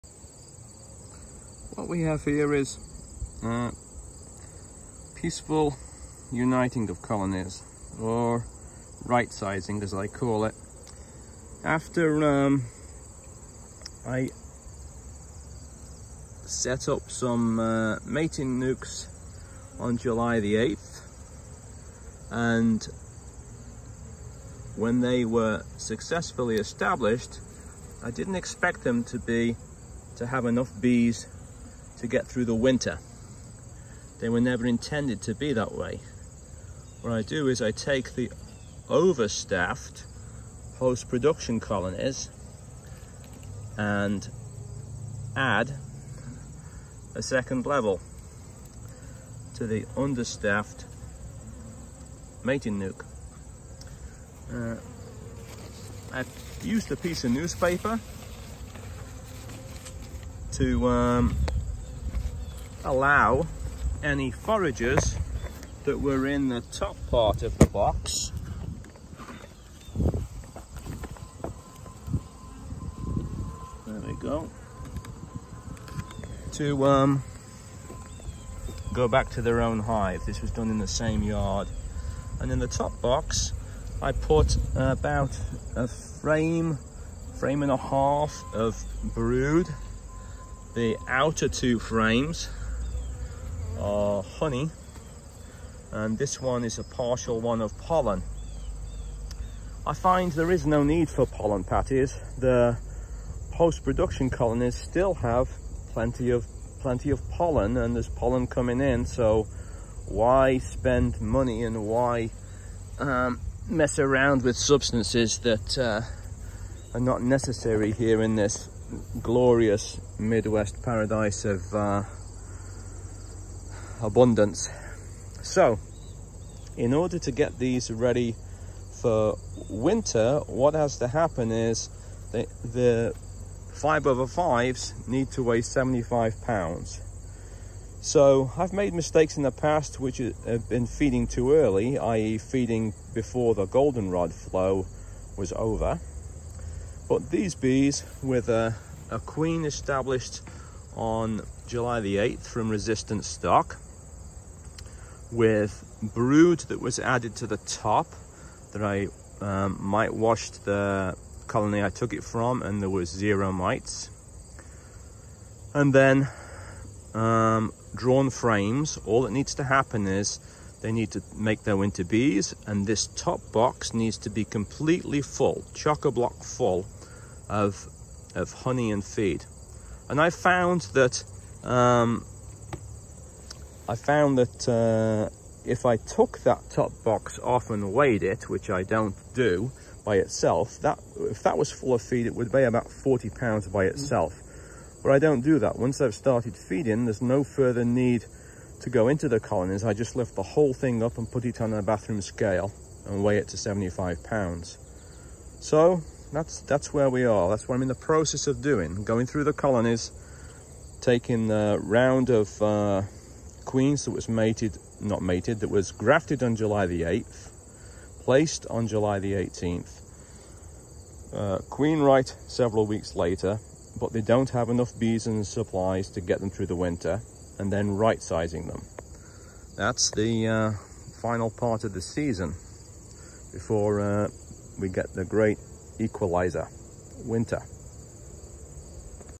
Rightsizing (boosting) understaffed colonies with overstaffed Post-Production colonies.
Colonies established in mating nucs early July are boosted with bees, brood, and honey from overstaffed Post-Production colonies.